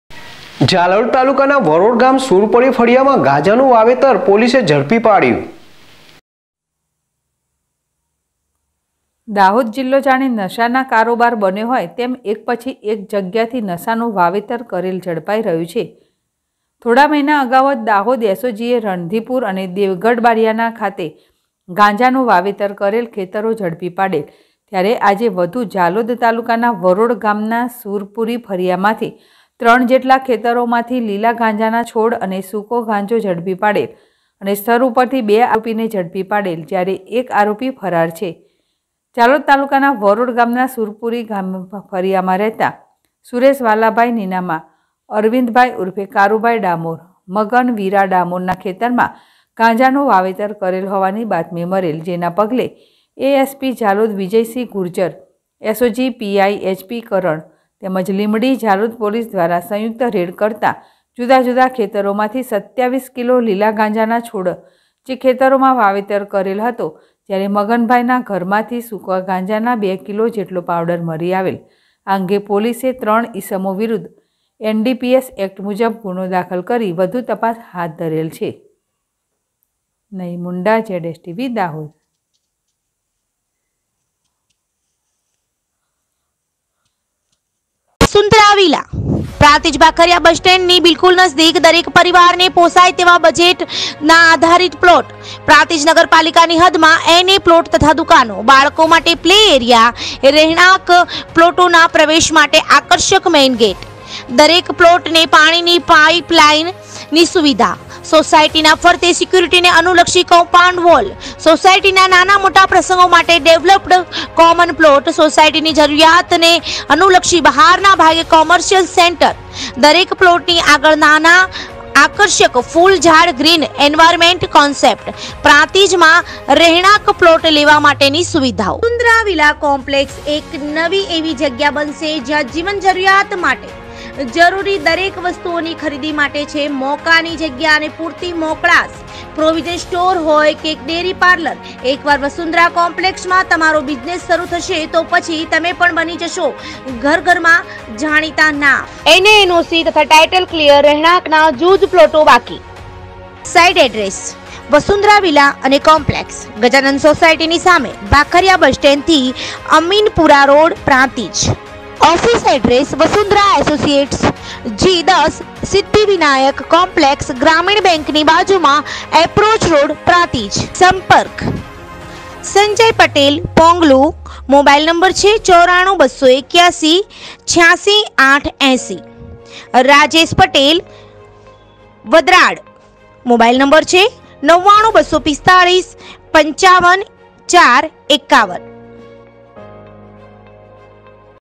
- ઝાલોદ તાલુકા ના વરોડ ગામ નામ સુરપળી ફળીયા મા નશા (ગાંજા) નુ વાવેતર પોલીસ એ ઝડપી પાડયુ
- ઝાલોદ તાલુકા ના વરોડ ગામ નામ સુરપળી ફળીયા મા નશા (ગાંજા) નુ વાવેતર પોલીસ એ ઝડપી પાડયુ//ZSTVNEWS//